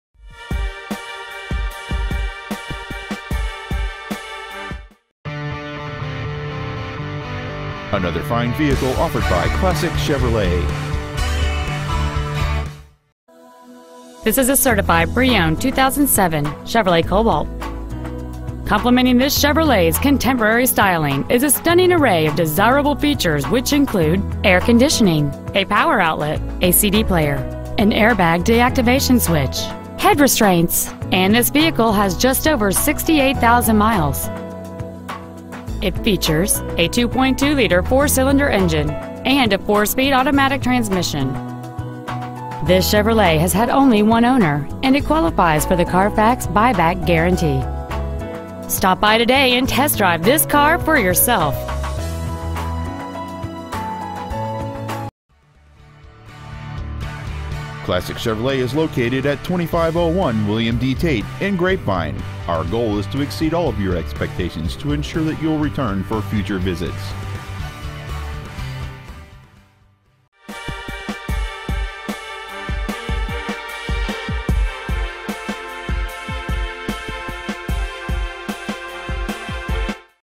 2007 Chevrolet Cobalt Grapevine TX
Year: 2007
 Make: Chevrolet
 Model: Cobalt
 Engine: Gas 4-Cyl 2.2L/134
 Trans.: Automatic
 Exterior: Gold
 Miles: 68,518
 Interior: Tan

 Preowned 2007 Chevrolet Cobalt Grapevine TX